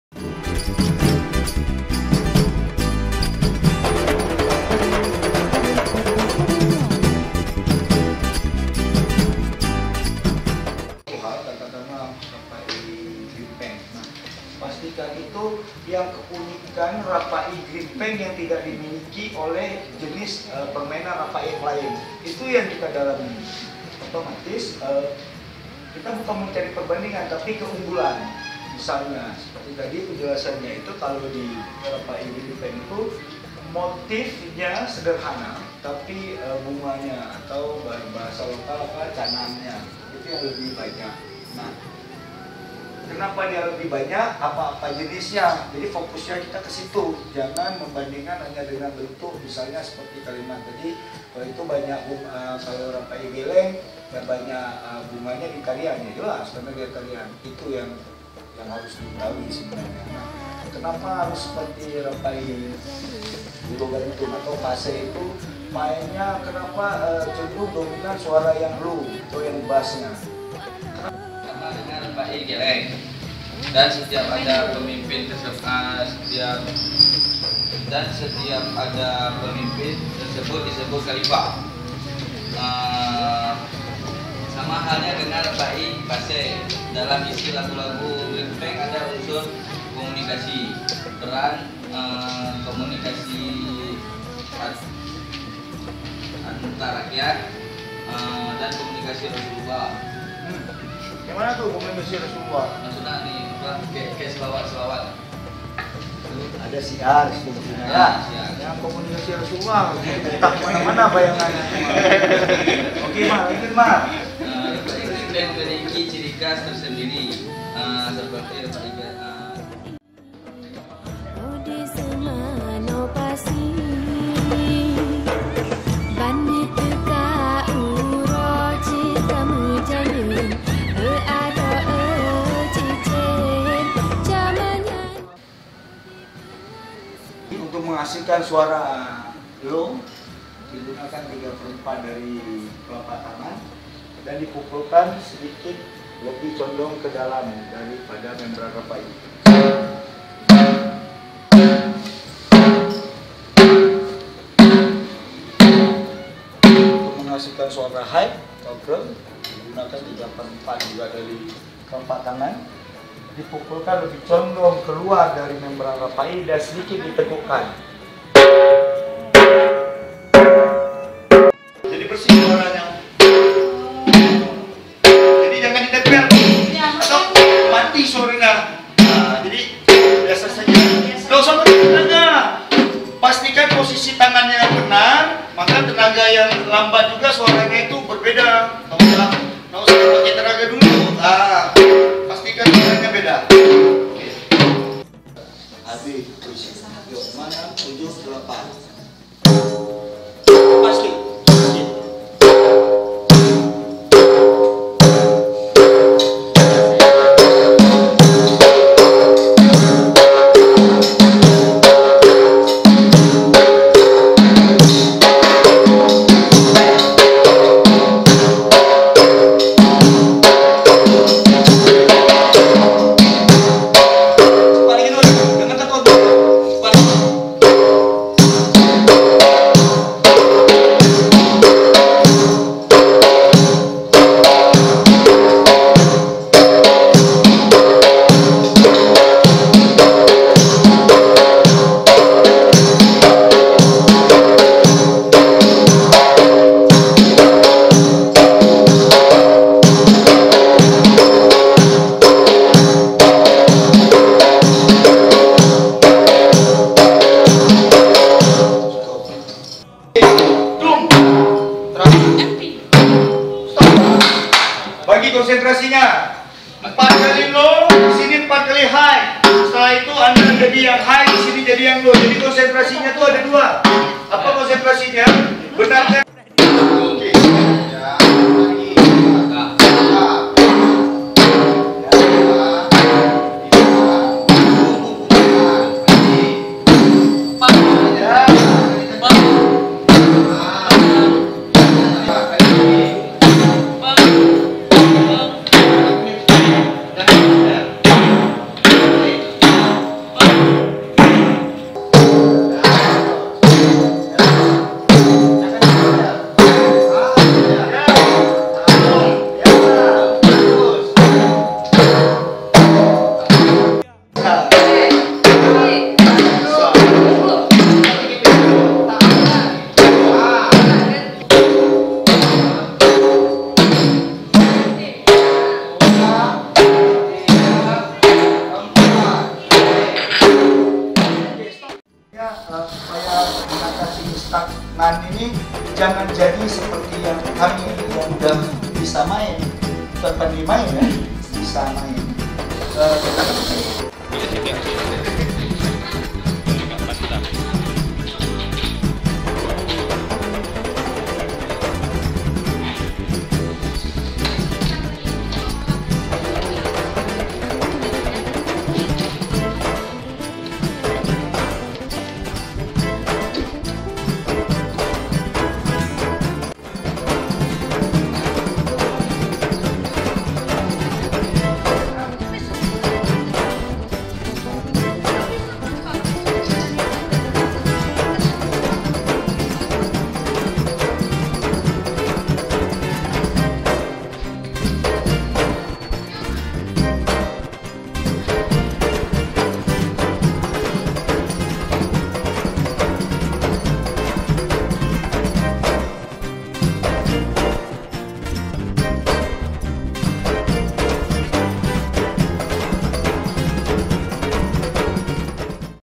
ayok belajar RAPAI ( PERKUSI ACEH ) , mahasiswa ISBI ACEH kuliah teori dan praktik rapai, HD 2017
mahasiswa ISBI ACEH kuliah teori dan praktik rapai di sanggar RAMPOE ACEH, maret 2017